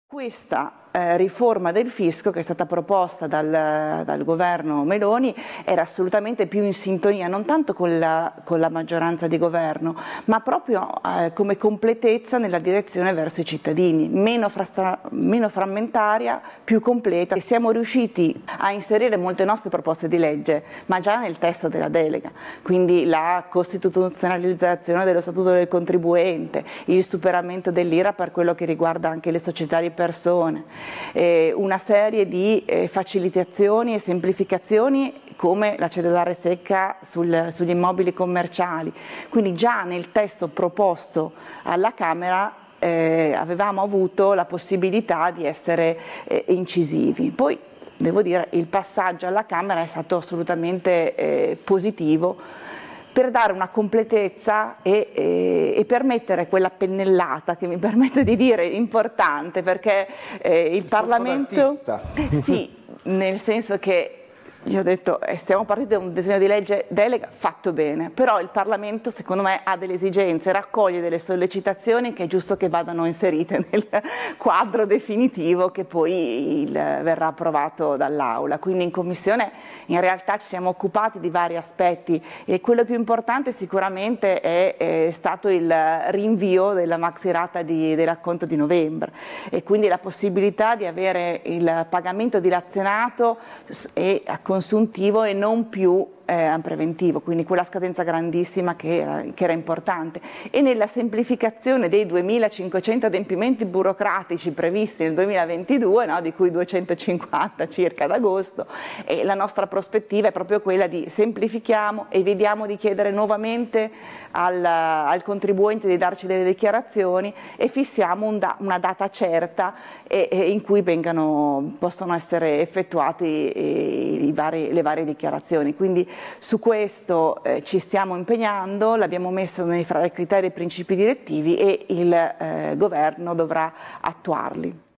Delega fiscale, Cavandoli (Lega): obiettivi chiari
Roma, 19 lug. (askanews) - "Le procedure della riscossione, il superamento dell'irap per le società di persone, una serie di facilitazioni come la cedolare
secca sugli immobili commerciali, sono tutti esempi di quanto possa essere efficace questo intervento. Tra gli aspetti più importanti, il rinvio della maxi rata dell'acconto di novembre con la possibilità di avere un pagamento dilazionato. La proposta di pace fiscale è per chi ha onorato tutti gli adempimenti fiscali ma non ha pagato. Ben 1.153 mld è il numero dei debiti
fiscali ad oggi, l'obiettivo è cercare di recuperare il più possibile e oggi solo il 10% può essere effettivamente riscosso. Di fronte a questi numeri cosa possiamo fare? Cercare di andare incontro a chi può pagare senza andare verso un condono. Partiamo da un problema enorme legato al tema della riscossione. L'obiettivo è renderla efficiente. Dobbiamo far emergere il nero
regolarizzando situazioni che oggi ci sfuggono", lo ha detto questa mattina l'On. Laura Cavandoli, membro della commissione Finanze della Camera, intervenuta a Largo Chigi, il format di The Watcher Post.
E sul salario minimo: "Nella valutazione di una riforma del lavoro, prima di pensare al salario minimo bisogna occuparsi di quelle fasce che lavorano in nero, con l'obiettivo di raggiungere la regolamentazione di queste situazioni e la rimodulazione delle tipologie di contratto. Tra l'altro, è notizia di oggi, è stato chiarito che per l'81% dei lavoratori il salario è ben più dei 9 /h richiesti dall'opposizione. La nostra riforma fiscale va in questa direzione, incentivando le persone a regolarizzarsi", ha concluso Cavandoli.